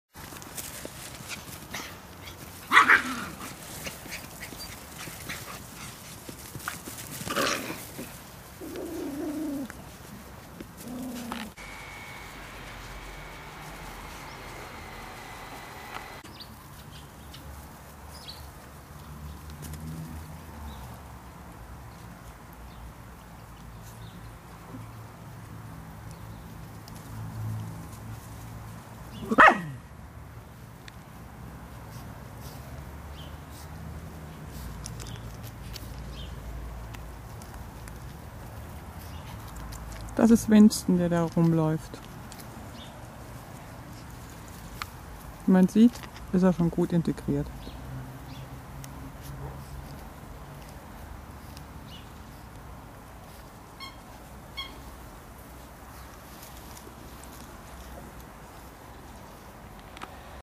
Winston will mehr
Winston ist erst seit 3 Tagen bei uns, doch er ist schon vom Steinwitz-Rudel integriert.
Immer wieder erstaunlich wie sozial die Hunde sind.
Da sollten sich einige Menschen ein Beispiel daran nehmen